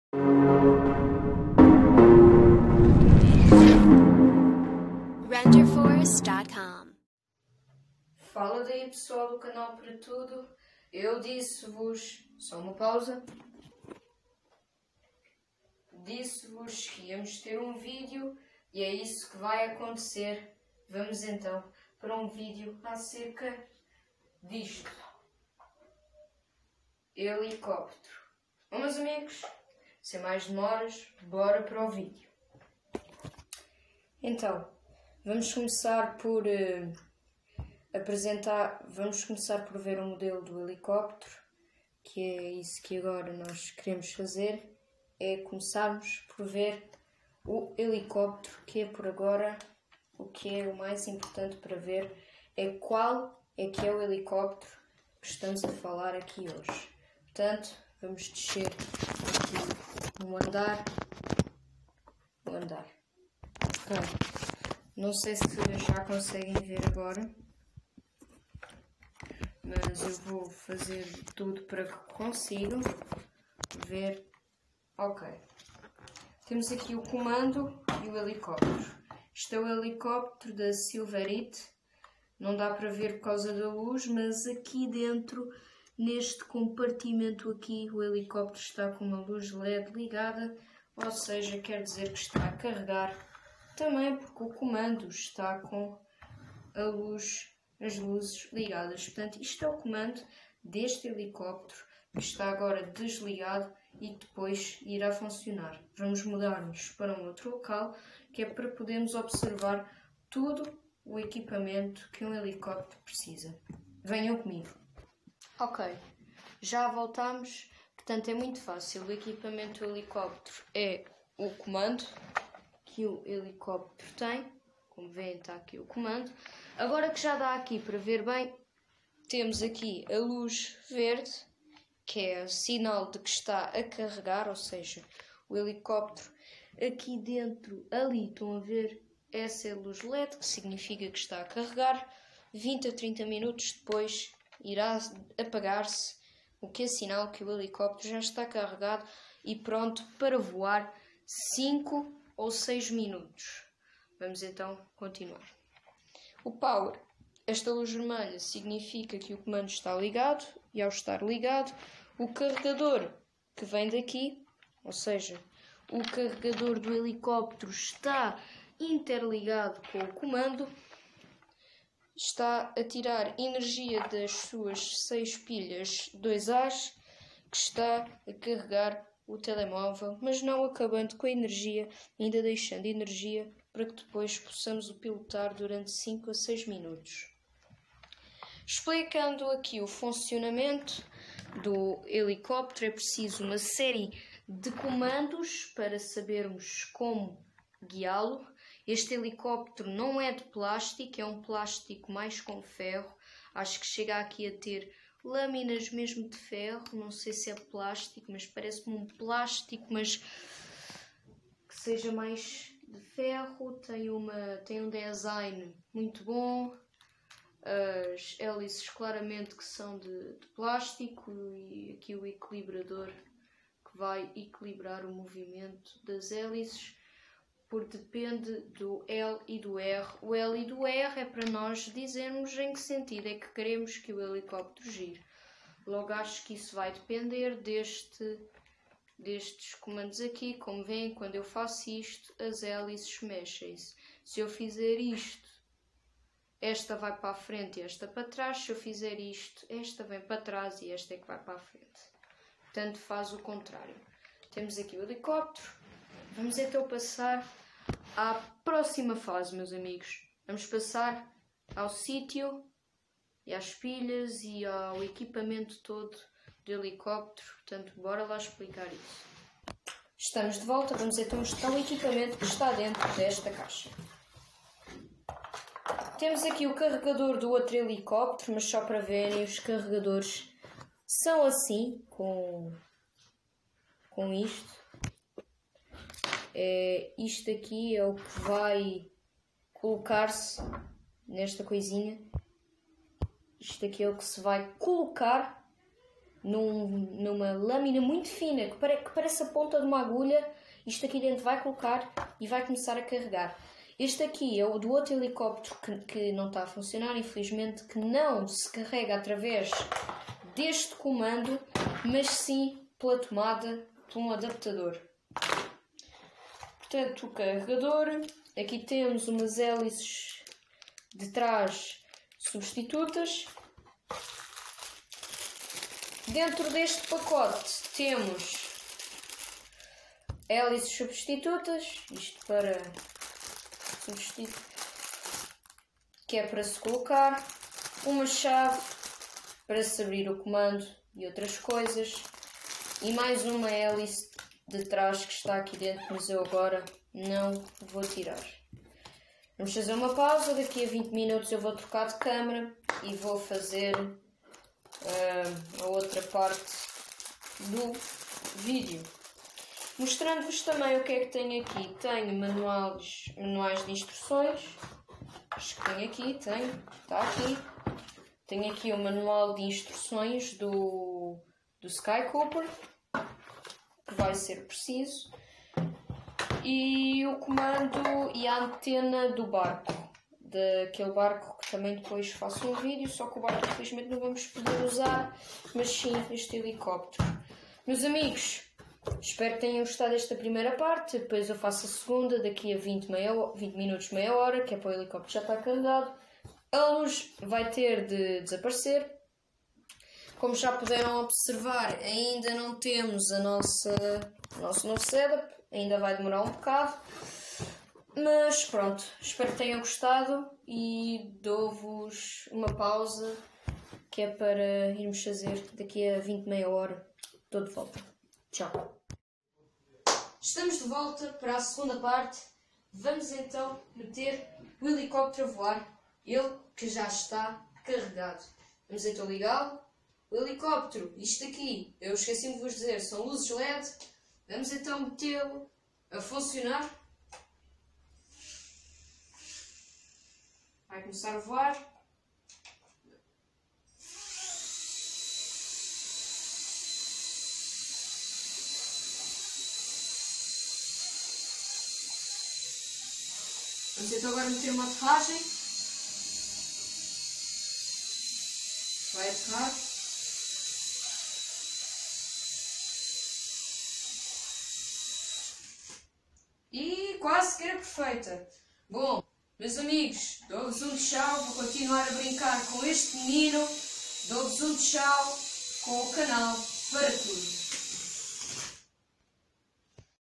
Mostrando e pilotando meu mini helicóptero! Com LUZES LED.
Neste video eu mostro e piloto meu mini helicóptero e falo sobre e a sua tecnologia.

Sigam-me nas redes

canal_para_tudo_yt